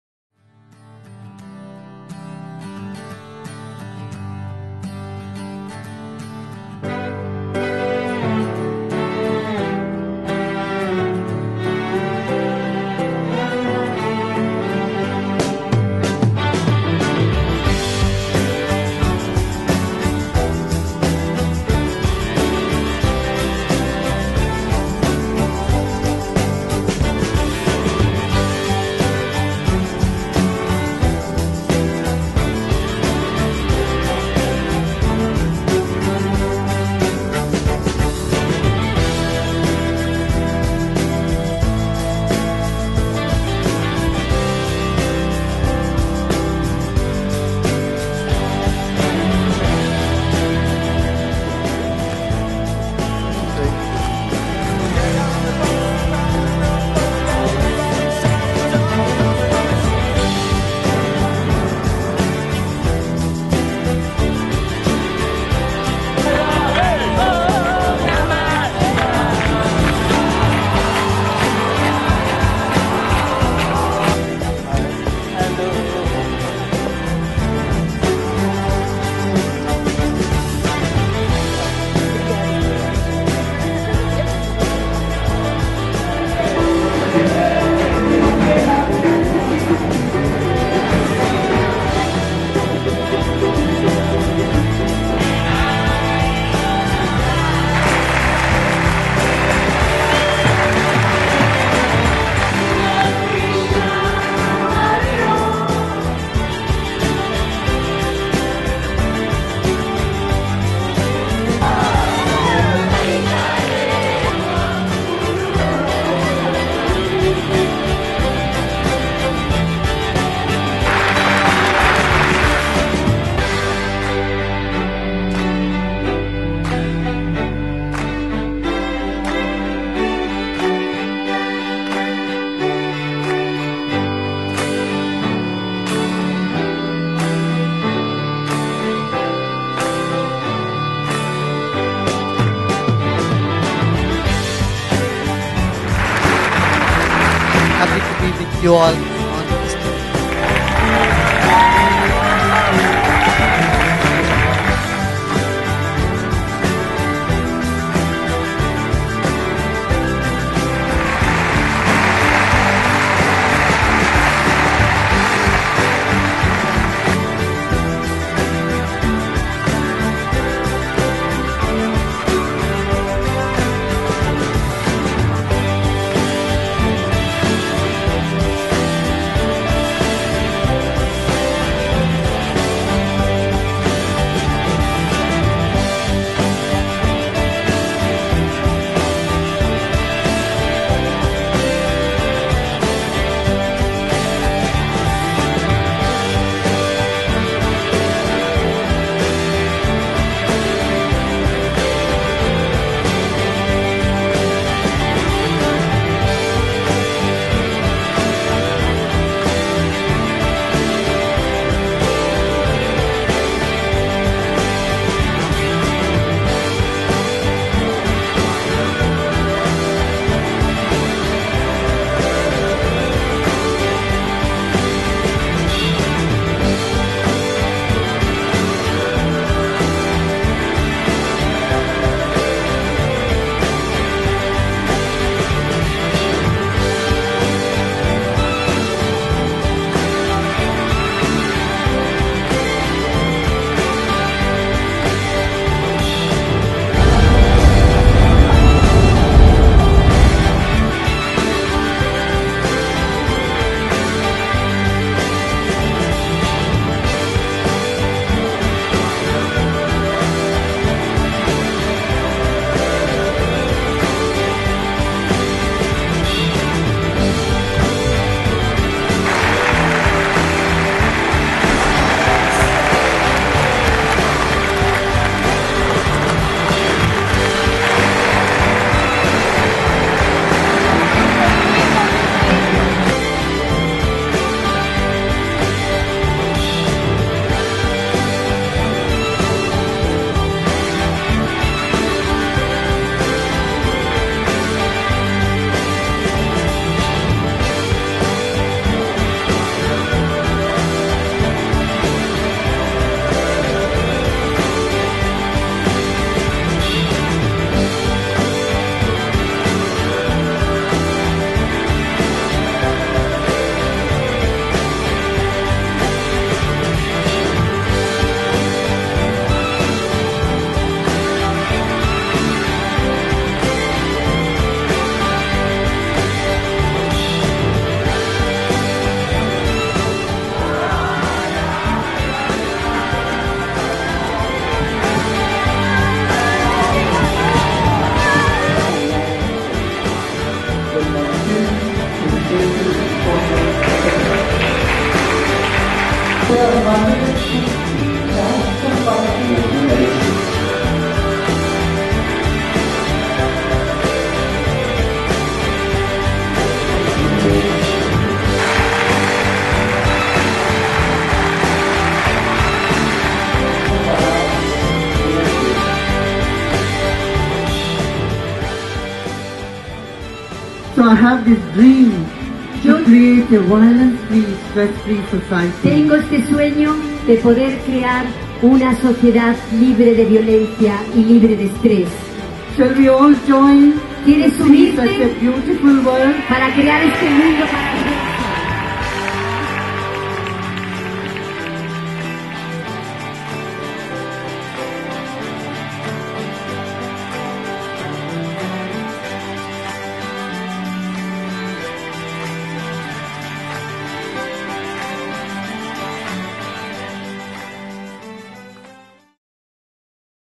O Amor Move o Mundo - América Latina 2012
Com a visão de uma sociedade livre de violência e de estresse, Sri Sri Ravi Shankar fundador da Fundação Arte de Viver inspira milhares de pessoas pelo mundo. Ele interpreta vários papéis para inúmeras pessoas -- como embaixador humanitário, líder espiritual ou professor de meditação, assim como defensor da paz.
 Ele que já foi indicado 4 vezes ao Prêmio Nobel da Paz têm escrito um novo capítulo da história da humanidade.

Vejam um pouco de como foi o tour de Sri Sri Ravi Shankar pela América Latina.